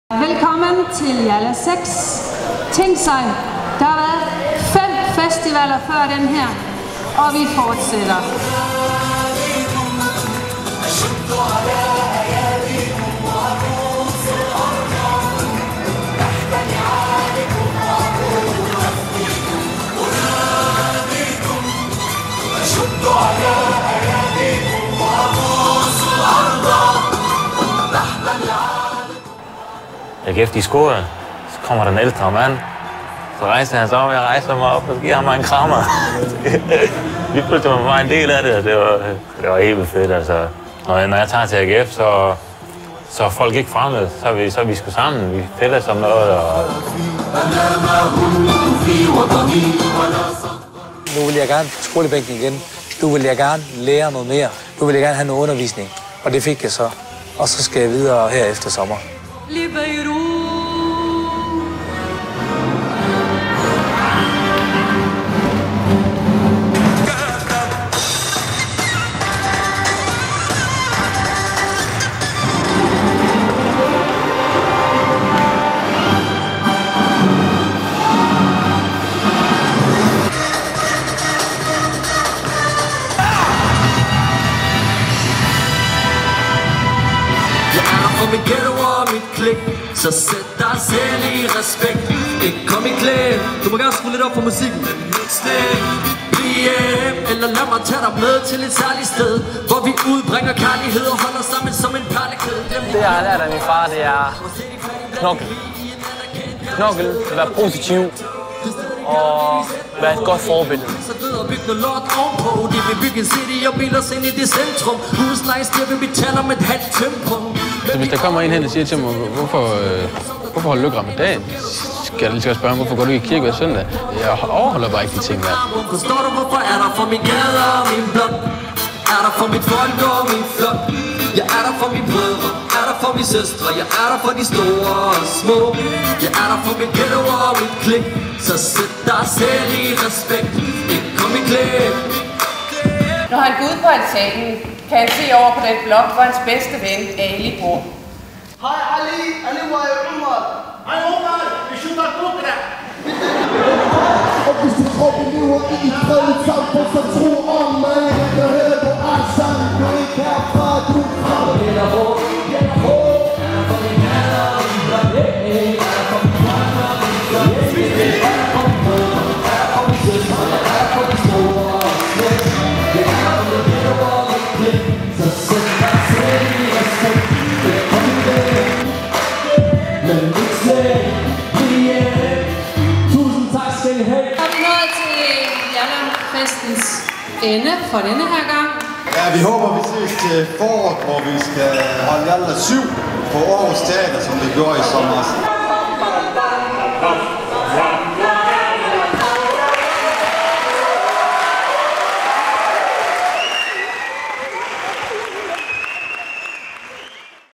Kulturfestivalen JALLA:6
JALLA:6 er den del af den række af gratis kulturfestivaler som OPGANG2 TURNÉTEATER afholder. JALLA:6 fandt sted på GLOBUS1 i Gellerup ,efteråret 2012 og bød blandt andet på forestillingen HVOR REGNBUEN ENDER, Marwan, Cha-D, Jafra-dans, elever fra Aarhus Teater og masser af musik.